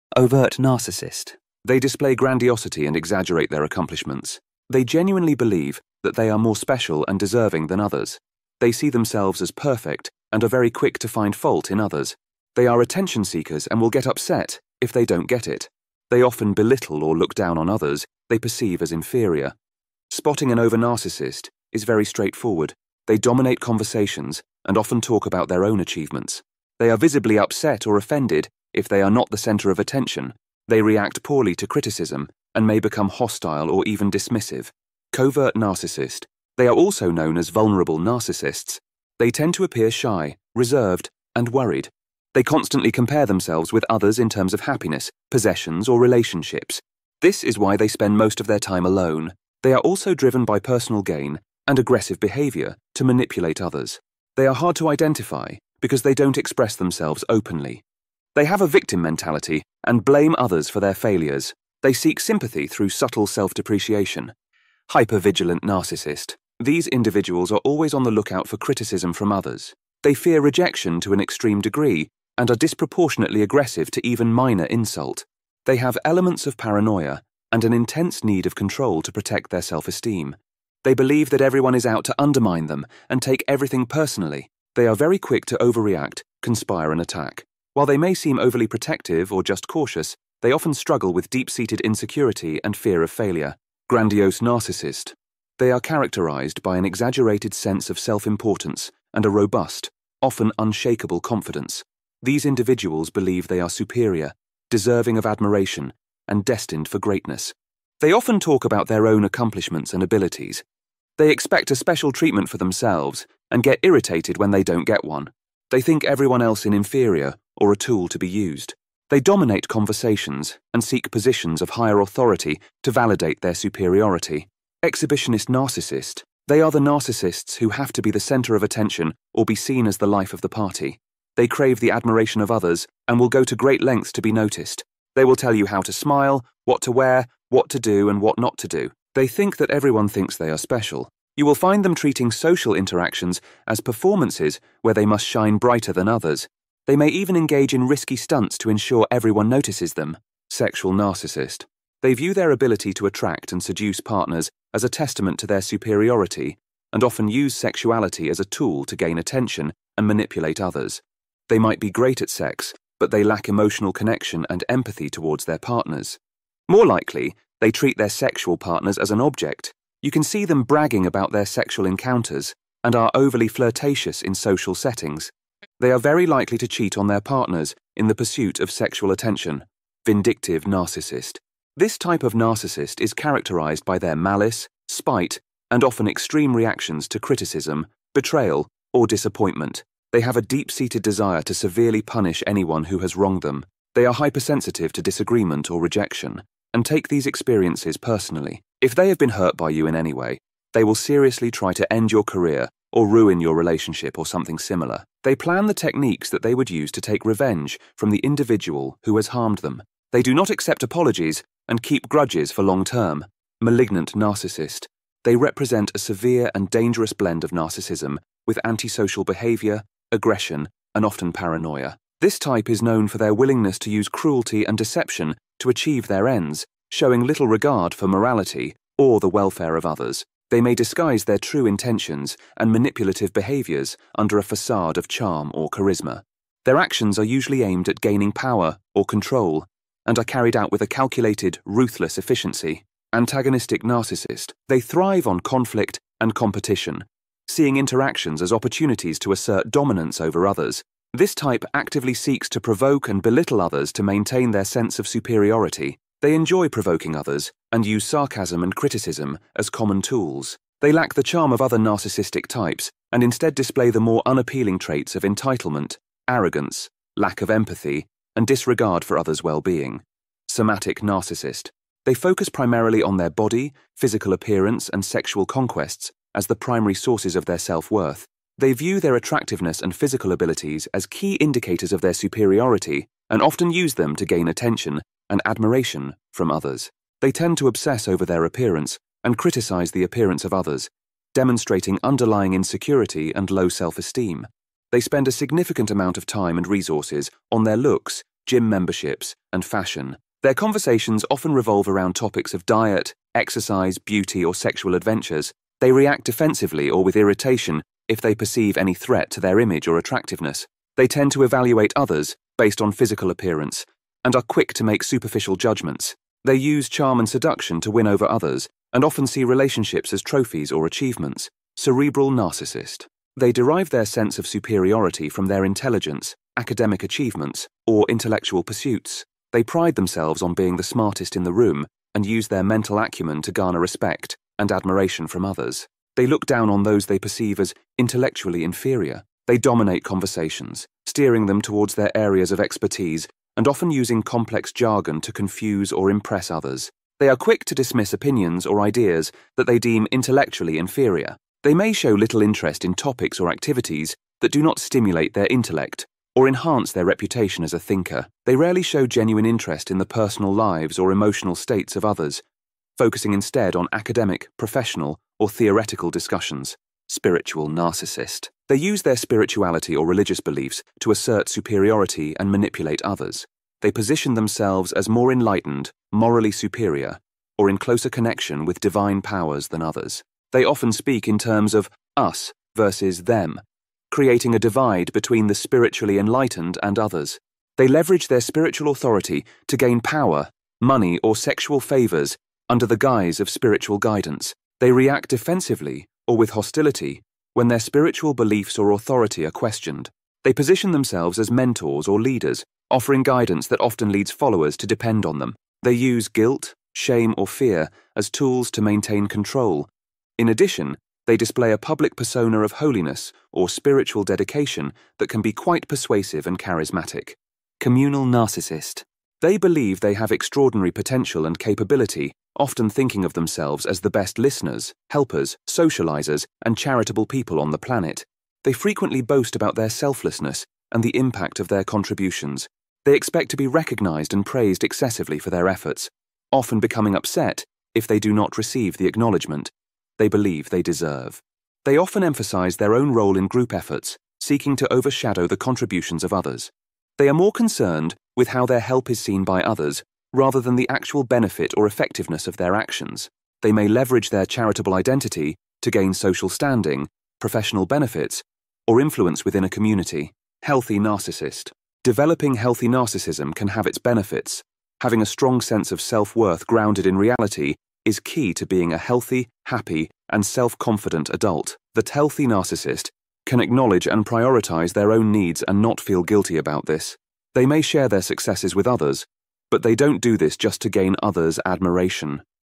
Every Type of Narcissist Explained in 8 minutes
This video is for reference only...

What kind of narcissist have you dealt with?

Explained in this video.